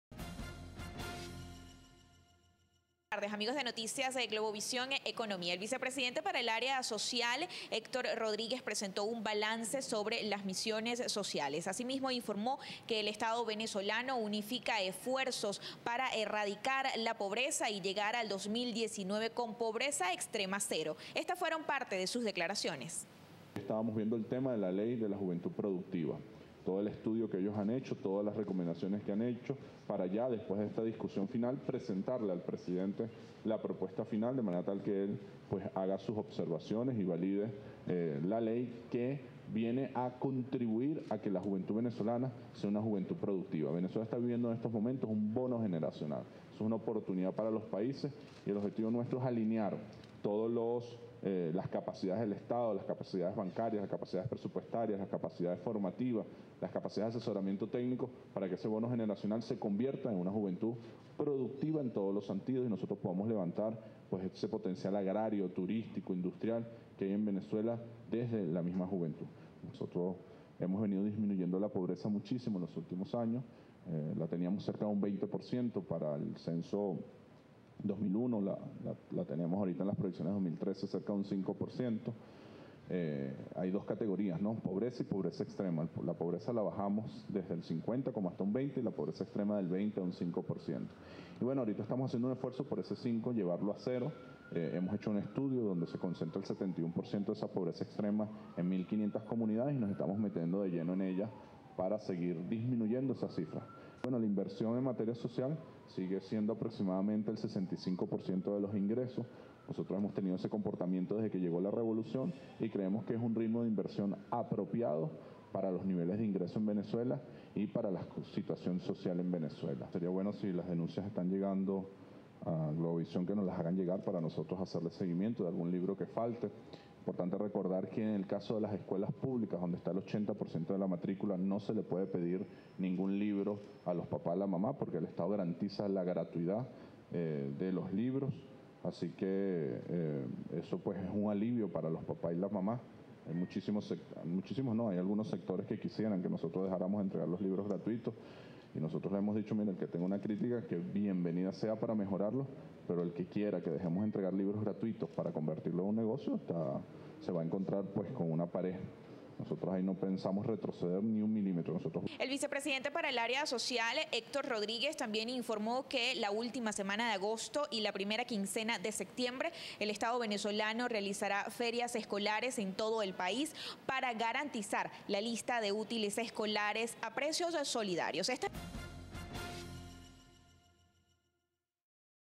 Entregarán propuesta final de Ley de Empleo Juvenil al presidente Maduro
El ministro para la Educación y vicepresidente para el Área Social, Héctor Rodríguez, informó este lunes que fue discutido el proyecto de Ley de Empleo Juvenil en el Gabinete Social y que será entregado en los próximos días al presidente de la República, Nicolás Maduro.